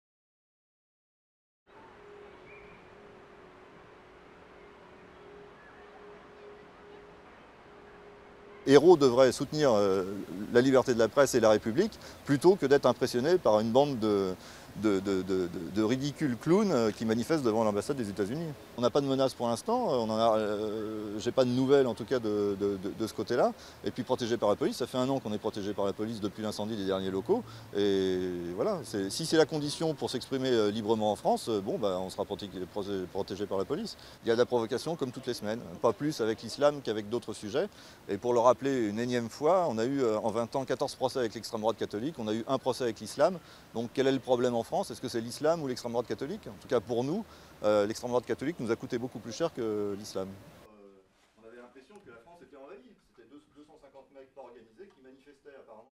Caricatures: Charb regrette l'attitude du gouvernement
Charb, le directeur de Charlie Hebdo qui publie des caricatures de Mahomet, estime mercredi que le Premier ministre devrait "soutenir la liberté de la presse et la République" plutôt que de se laisser impressionner par "des clowns ridicules qui manifestent" devant l'ambassade américaine à Paris. Durée: 00:55